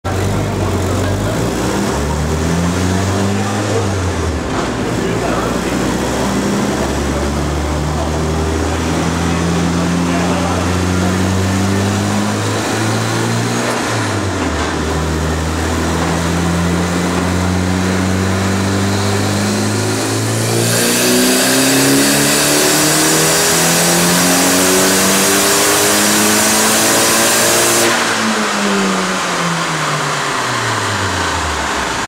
gtsiawd96 1995 Eagle Talon TSi awd 307 awhp dyno run 2
11/28/09 psi proformance dyno run 2
307hp 285tq
6 bolt engine swap
Dsmlink
Big 16g turbo at 23psi
Intake and filter
o2 dump
Ported 2g exhaust mani
3" Apexi N-1 turboback
SSAC fmic
Greddy RS bov
Coil on plug ignition setup
750cc injectors
walboro 255 fuel pump